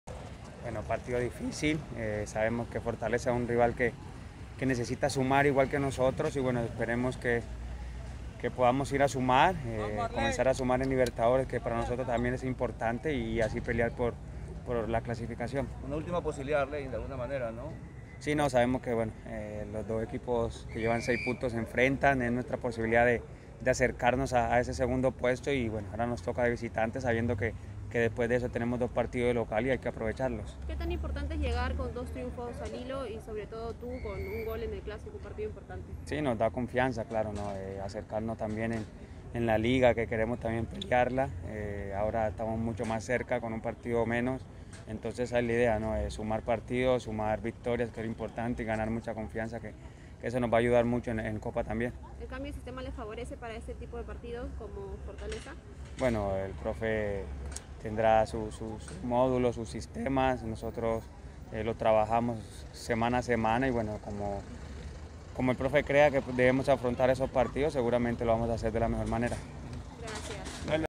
Arley Rodríguez: "Queremos sumar en la Libertadores"
El delantero de Alianza Lima indica que llegó la hora de ganar en la Libertadores.